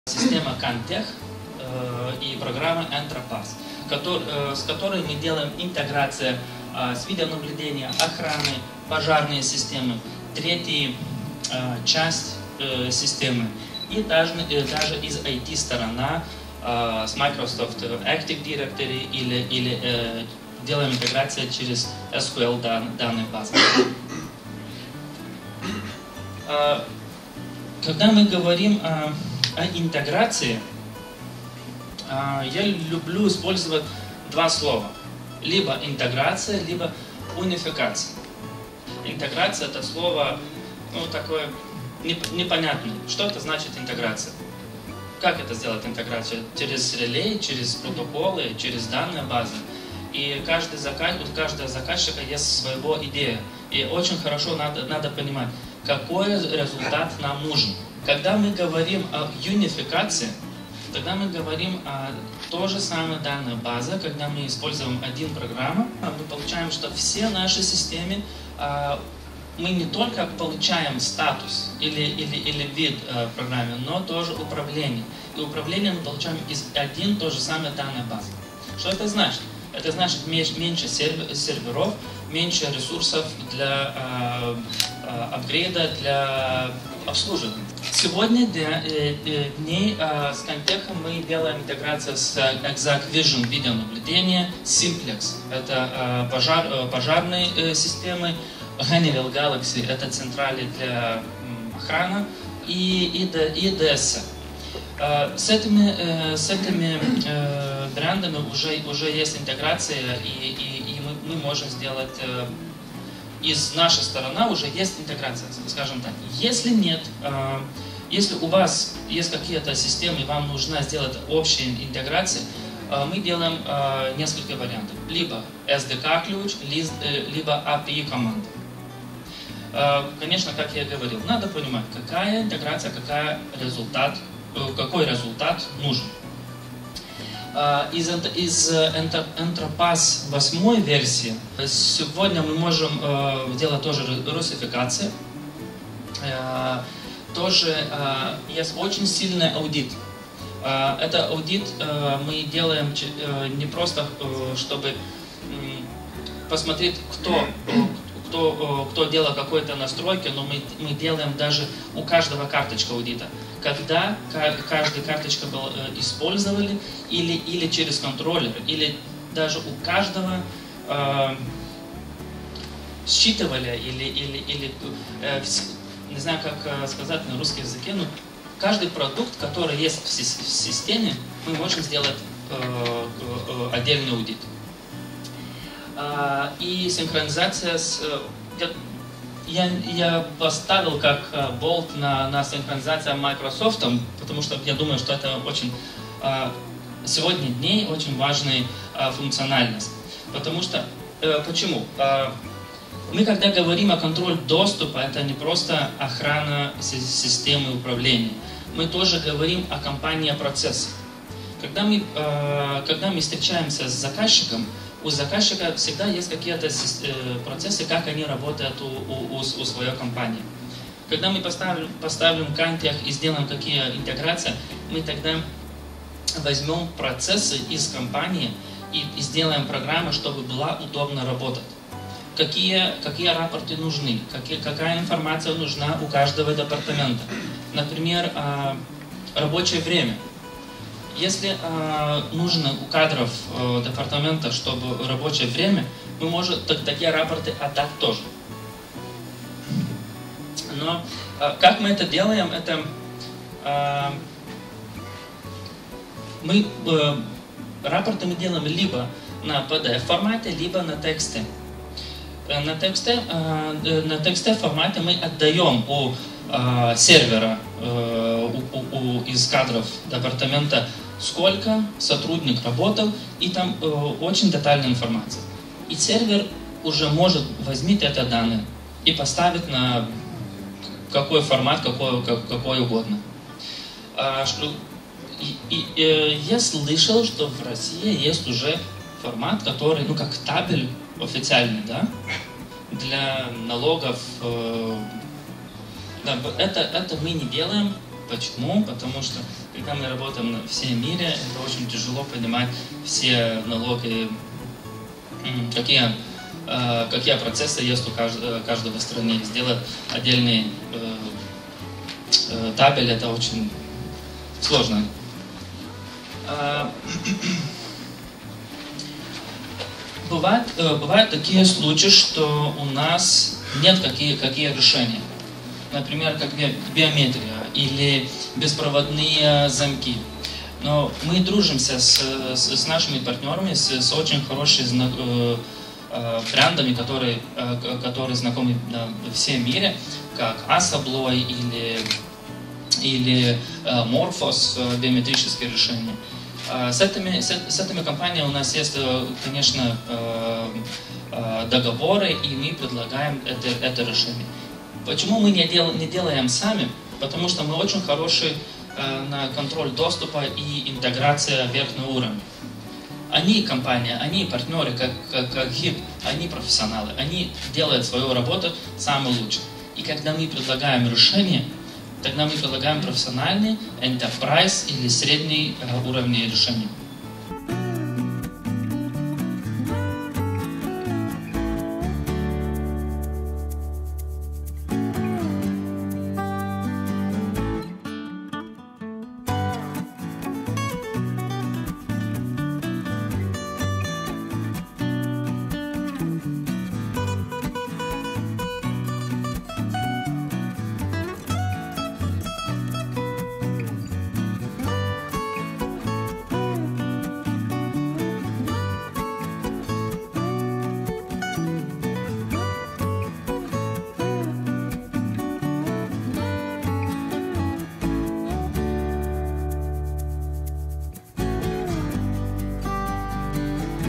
СКУД Kantech. Варианты интеграции
Aurimas Sukys "Kantech"
Система Kantech и программа EntraPass, с которой мы делаем интеграцию с видеонаблюдением, охранной, пожарной системой, другими системами и даже с IT-решениями с Microsoft Active Directory или делаем интеграцию через SQL DataBase.
Когда мы говорим об интеграции, я люблю использовать два слова:
- либо "интеграция",
- либо "унификация".
Интеграция – это слово, ну, такое, непонятное: что это значит – интеграция? Как это сделать интеграцию? Через реле? Через протоколы? Через базу данных?
И у каждого заказчика есть своя идея. Очень хорошо надо понимать, какой результат нам нужен.

Обсудить на Форуме "Моста Безопасности" в теме "СКУД. Знать, что хочет заказчик"